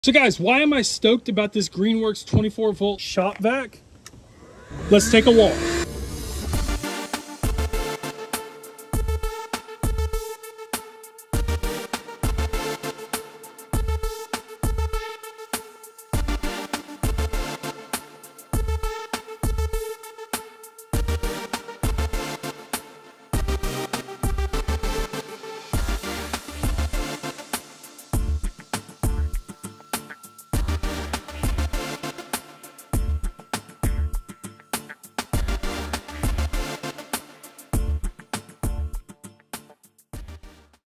Greenworks 24V Cordless Shop-Vac
The Greenworks 24V Cordless Shop-Vac can vacuum where you want, when you want and what you want!